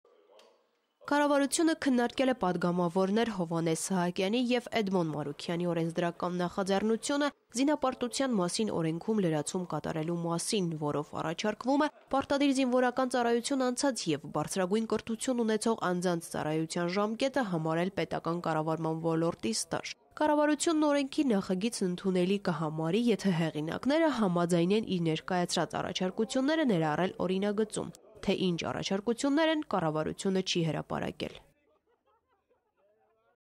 Կառավարությունը քննարկել է "Զինապարտության մասին" օրենքում լրացում կատարելու հարցը
Կառավարությունը քննարկել է պատգամավորներ Հովհաննես Սահակյանի եւ Էդմոն Մարուքյանի օրենսդրական նախաձեռնությունը "Զինապարտության մասին" օրենքում լրացում կատարելու մասին"